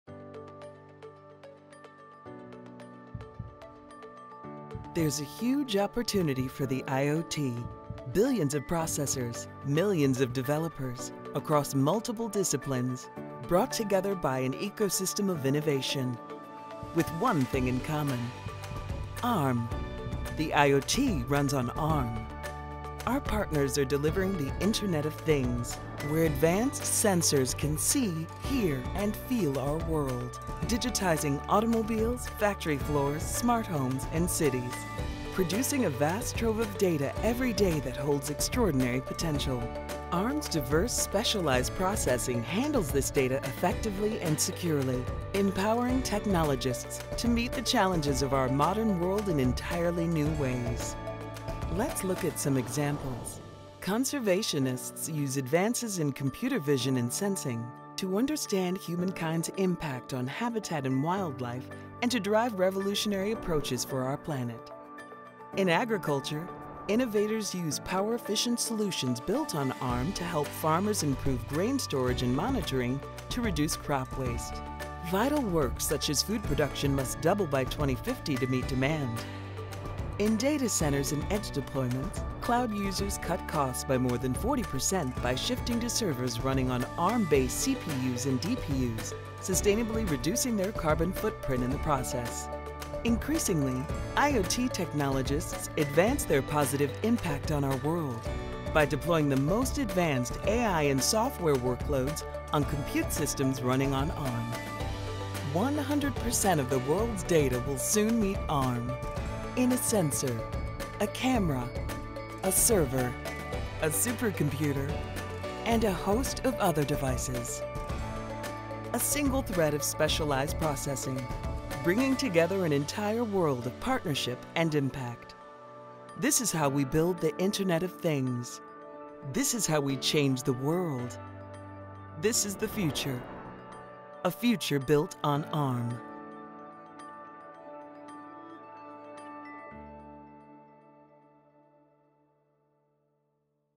The IoT Runs on Arm ー IoTを支える、Armで
IoTは、世界を変えています。数十億のデバイスが膨大な新しいデータソースを自動的に収集・分析し、人間とデジタル両方の意思決定を変革する可能性をそなえています。

ArmはIoTを支えています。ソフトウェア開発者コミュニティや、重要なIoT標準を中心に協業するエコシステムとともに、エンドポイントからクラウドソリューションの安全かつ摩擦のない展開をサポートします。

IoTの未来はここにあります。未来を築く、Armで

00:00 IoTの大きな可能性
00:23 IoTが産業全体にもたらす変革
00:53 環境保護家によるIoTを活用した革新的なアプローチ
01:05 農業分野で穀物の貯蔵や農業廃棄物の削減を支援
01:20 データセンターやエッジで、サーバーにArmベースのCPUやDPUを導入することで40％以上のコストを削減
01:45 あらゆる世界のデータはArmで動くように
2:13 IoTの未来を築く、Armで